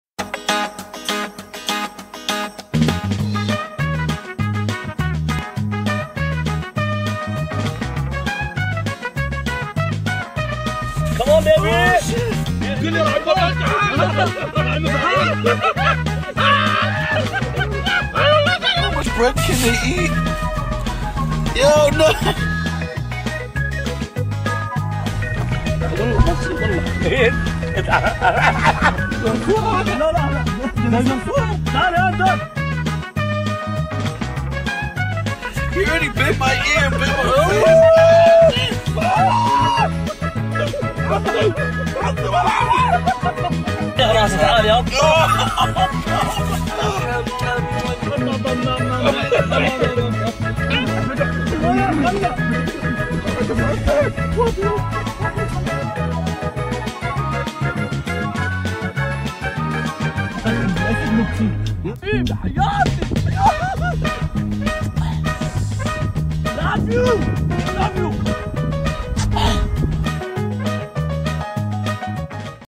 Cet Emirati fait peur à ses amis ! 🐫😃
Cet Emirati fait peur à ses amis... à l'aide de chameaux ! ⁠🐫😃